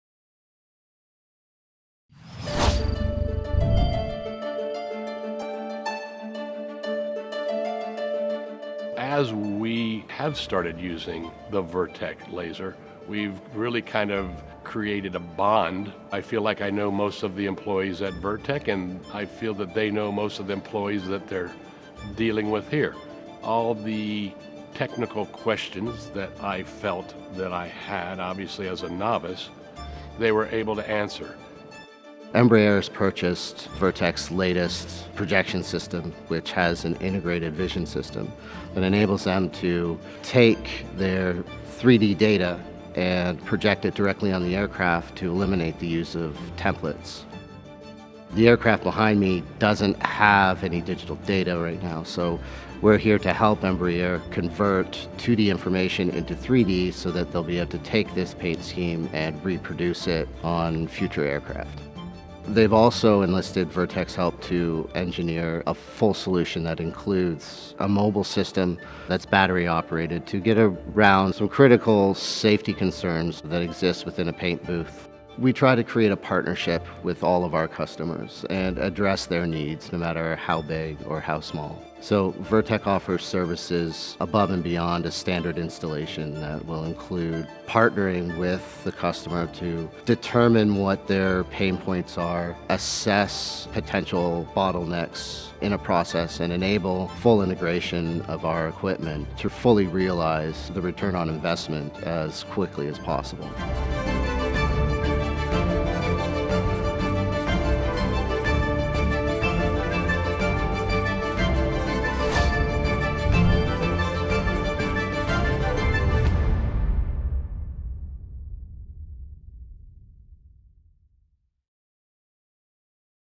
Virtek Embraer Service Project
The Virtek applications and service team worked with the Embraer Paint Team to carry out the digitizing, post-processing and projection file creation to deliver a paint-masking digital laser template file. Essentially we helped convert the 2D paint scheme into a 3D digital file so they can reproduce this paint scheme easily and use it on future aircraft's.